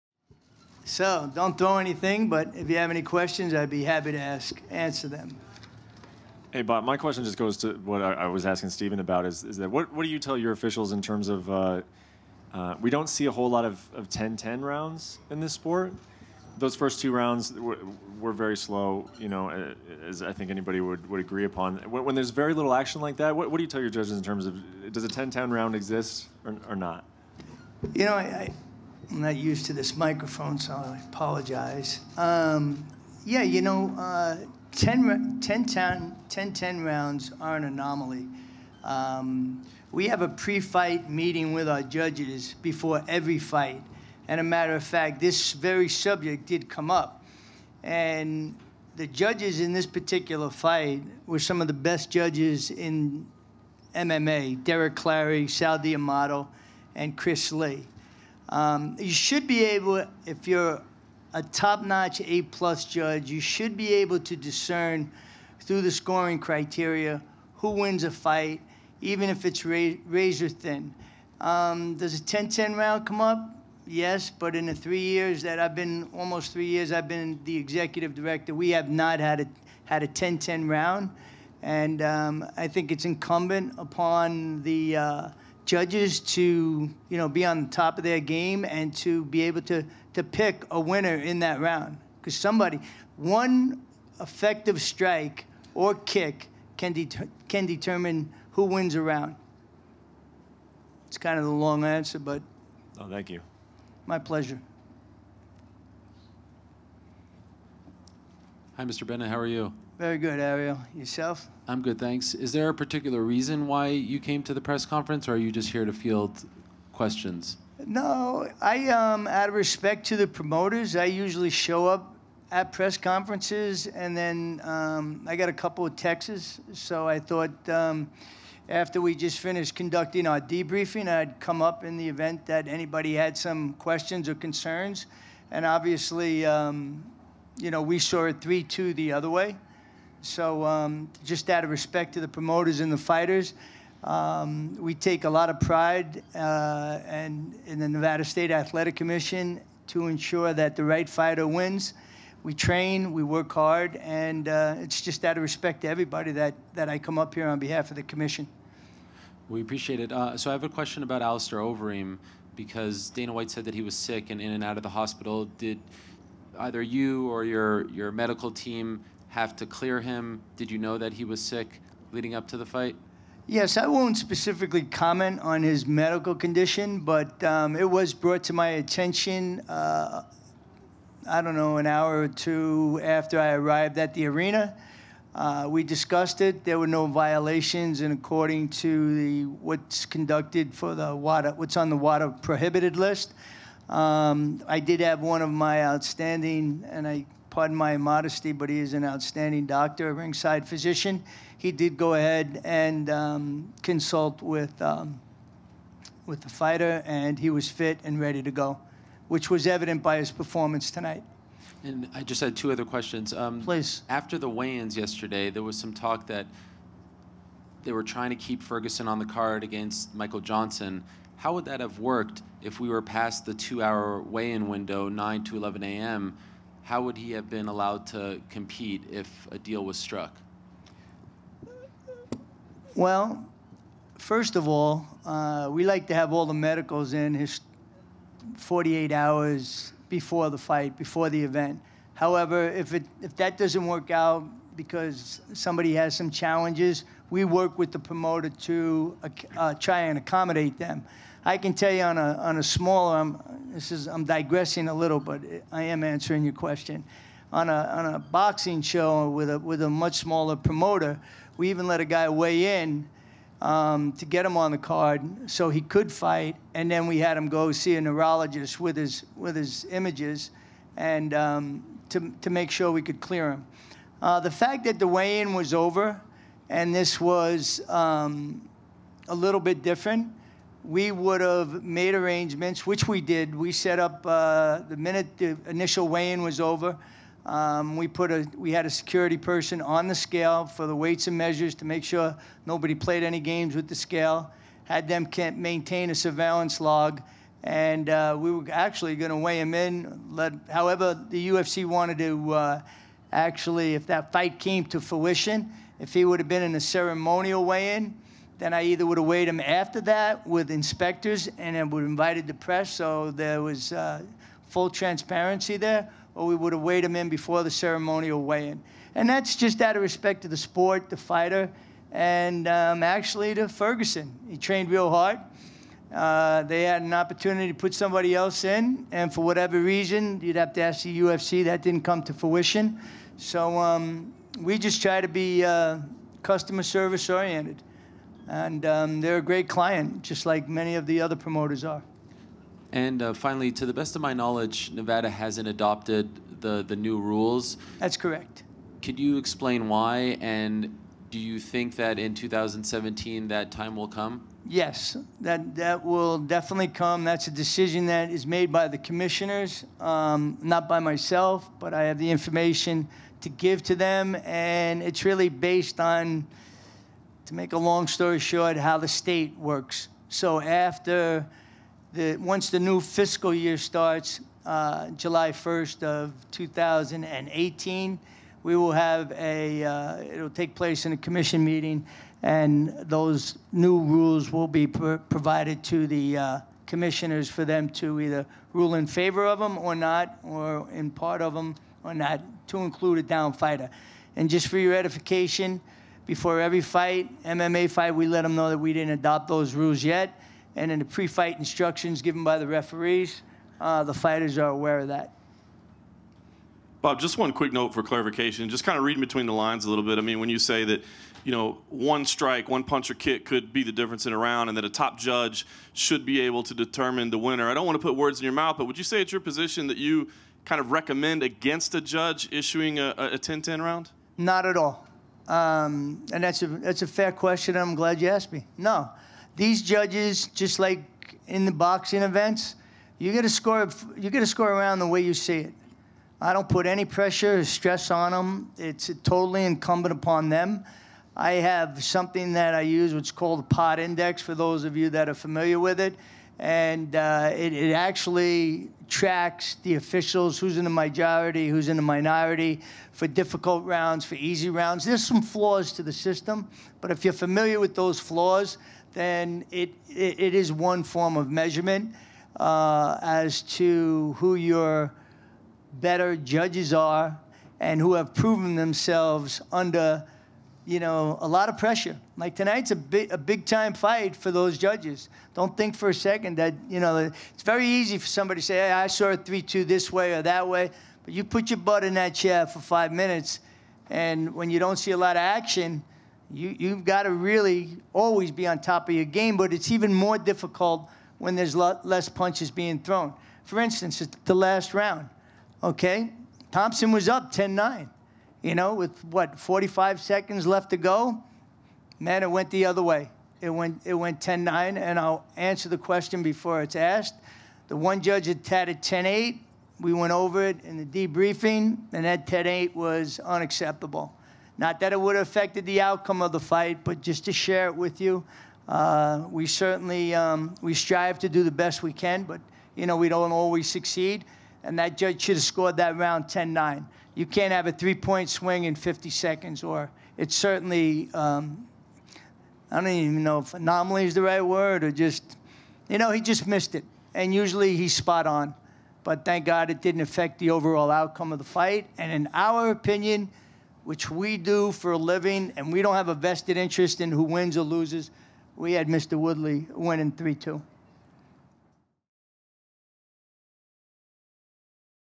NSAC head Bob Bennett talks UFC 209 scoring, what judges got wrong
NSAC Executive Director Bob Bennett talks UFC 209 scoring, including what judges got wrong in the main event, as well as how Tony Ferguson could have possibly faced Michael Johnson.